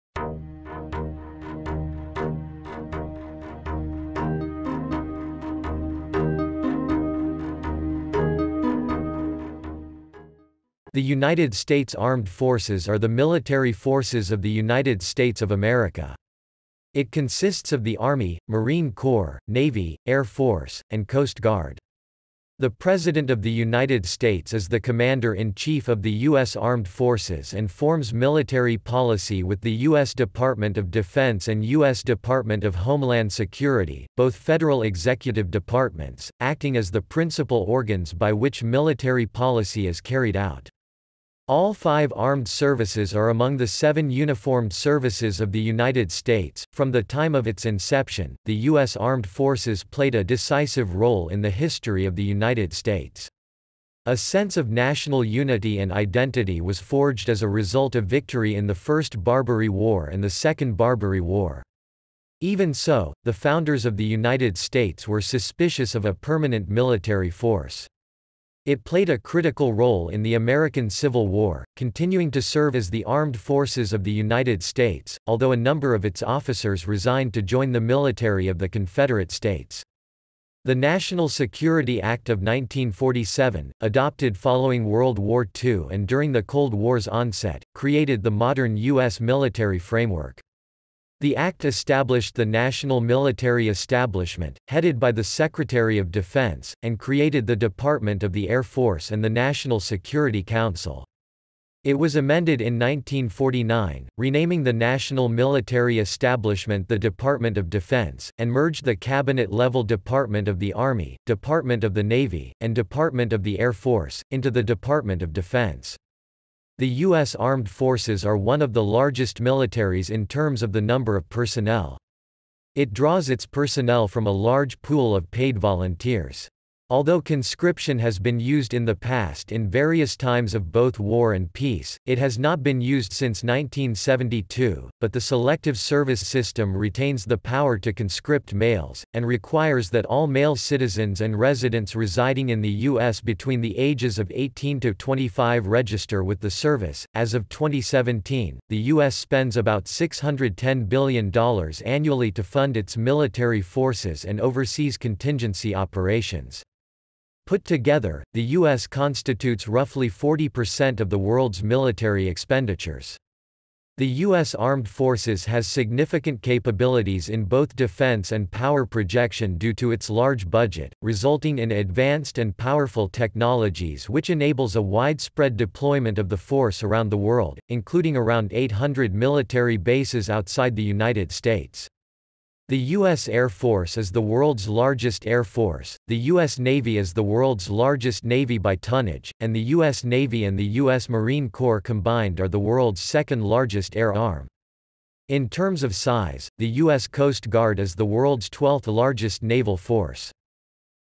What is United States Armed Forces?, Explain United States Armed Forces
~~~ United States Armed Forces ~~~

Title: What is United States Armed Forces?, Explain United States Armed Forces

Description: The United States Armed Forces are the military forces of the United States of America. It consists of the Army, Marine Corps, Navy, Air Force, and Coast Guard. The President of the United States is the Commander-in-Chief of the U.S. Armed Forces and forms military policy with the U.S. Department of Defense and U.S. Department of Homeland Security , both federal executive departments, acting as the principal organs by which military policy is carried out. All five armed services are among the seven uniformed services of the United States.From the time of its inception, the U.S. Armed Forces played a decisive role in the history of the United States. A sense of national unity and identity was forged as a result of victory in the First Barbary War and the Second Barbary War. Even so, the founders of the United States were suspicious of a permanent military force. It played a critical role in the American Civil War, continuing to serve as the armed forces of the United States, although a number of its officers resigned to join the military of the Confederate States. The National Security Act of 1947, adopted following World War II and during the Cold War's onset, created the modern U.S. military framework. The Act established the National Military Establishment, headed by the Secretary of Defense; and created the Department of the Air Force and the National Security Council. It was amended in 1949, renaming the National Military Establishment the Department of Defense, and merged the cabinet-level Department of the Army, Department of the Navy, and Department of the Air Force, into the Department of Defense. The U.S. Armed Forces are one of the largest militaries in terms of the number of personnel. It draws its personnel from a large pool of paid volunteers. Although conscription has been used in the past in various times of both war and peace, it has not been used since 1972, but the Selective Service System retains the power to conscript males, and requires that all male citizens and residents residing in the U.S. between the ages of 18–25 register with the service.As of 2017, the U.S. spends about US billion annually to fund its military forces and Overseas Contingency Operations. Put together, the U.S. constitutes roughly 40 percent of the world's military expenditures. The U.S. Armed Forces has significant capabilities in both defense and power projection due to its large budget, resulting in advanced and powerful technologies which enables a widespread deployment of the force around the world, including around 800 military bases outside the United States. The U.S. Air Force is the world's largest air force, the U.S. Navy is the world's largest navy by tonnage, and the U.S. Navy and the U.S. Marine Corps combined are the world's second largest air arm. In terms of size, the U.S. Coast Guard is the world's 12th largest naval force.